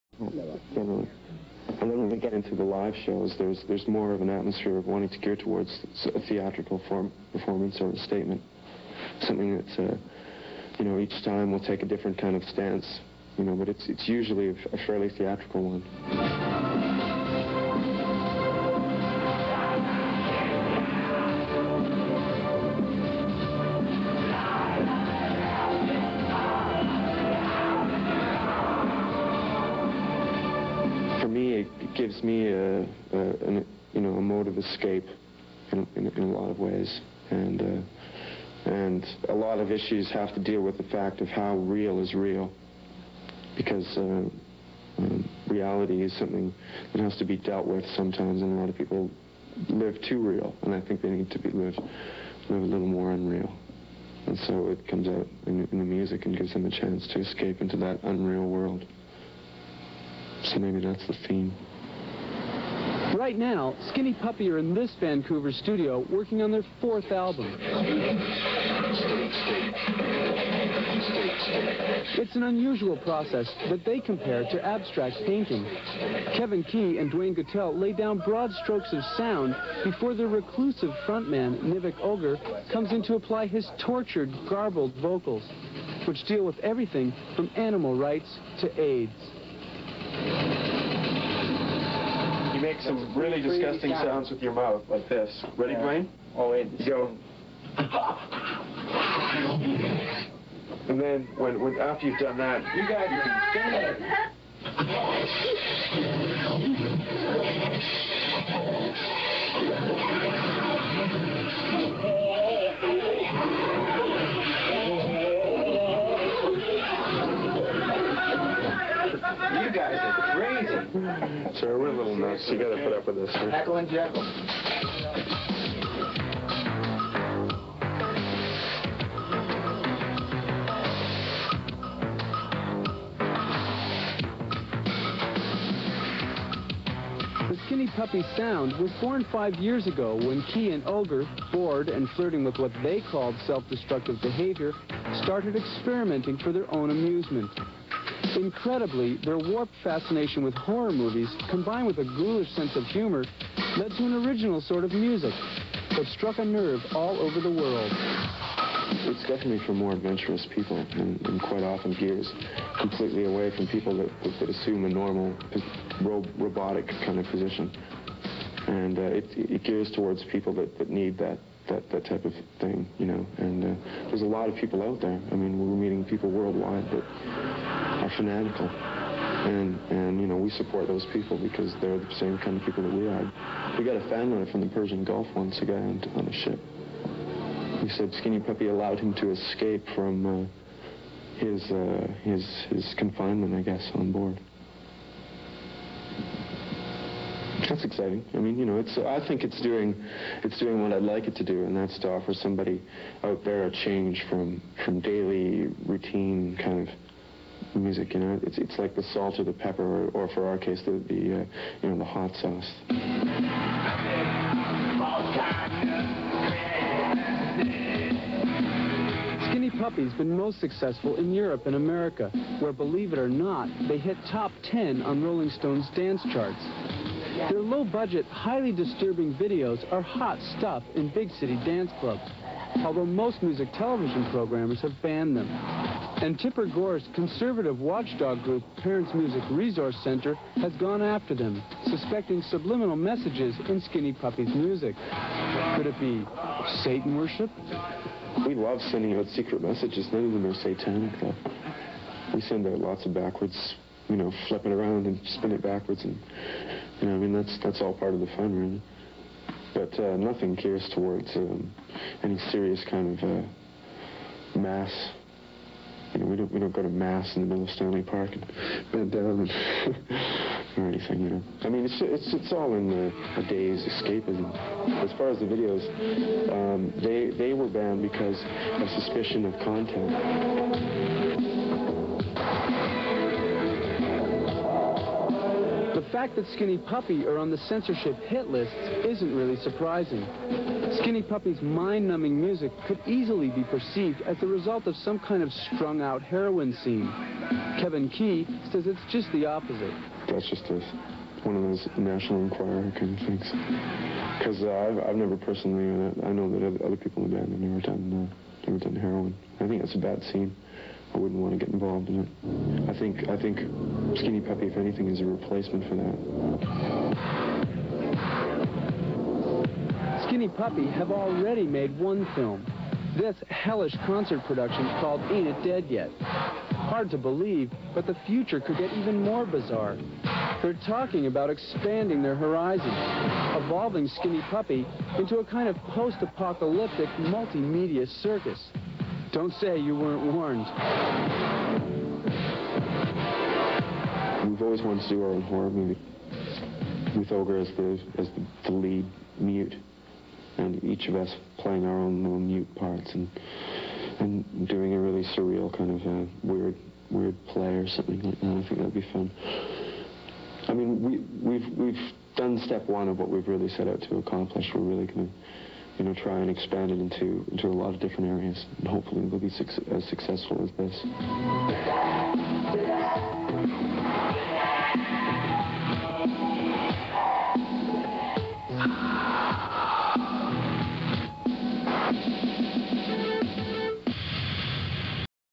Wired Interview cEvin Key 1988
Skinny Puppy Interview.  This is known as the "Mushroom Studios" interview.

Highlights include: Puppy in the recording studio.